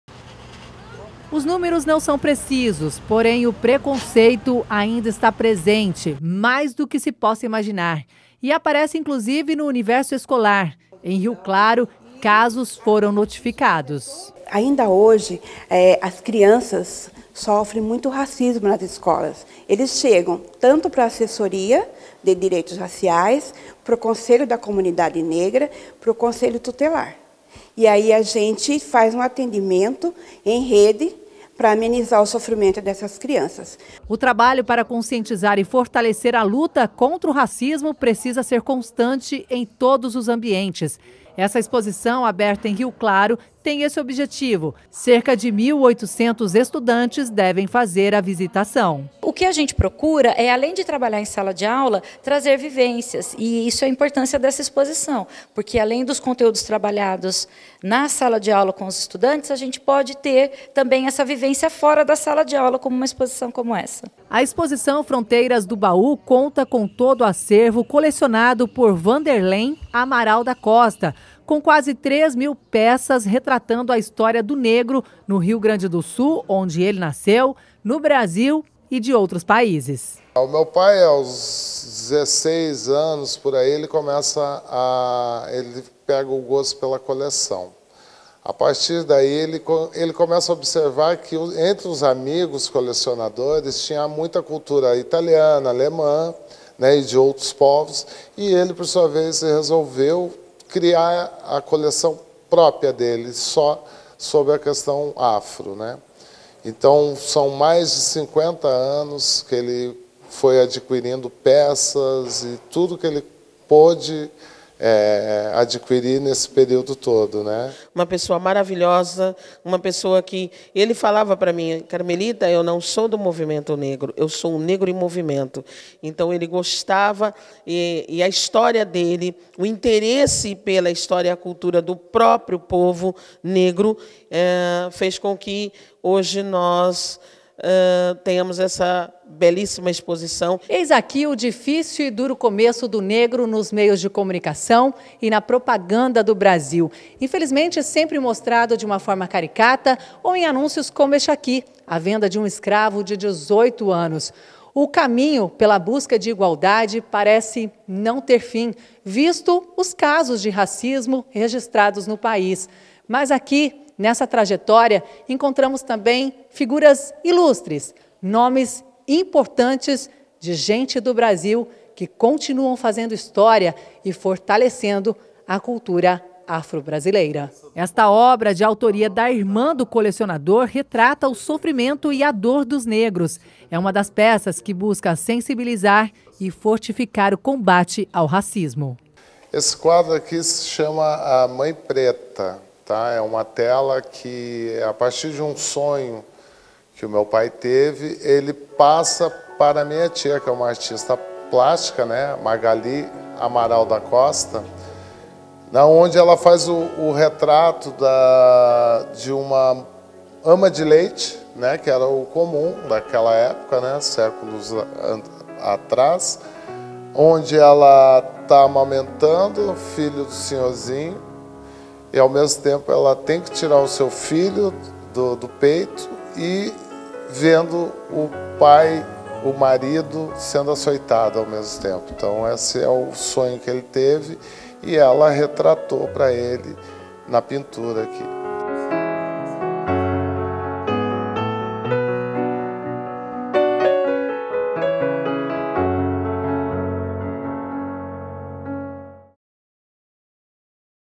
Rio Claro realiza exposição Fronteiras do Baú
O município de Rio Claro realiza neste mês a exposição Fronteiras do Baú, que poderá ser visitada do dia 13 ao 27, no Museu Histórico e Pedagógico Amador Bueno da Veiga.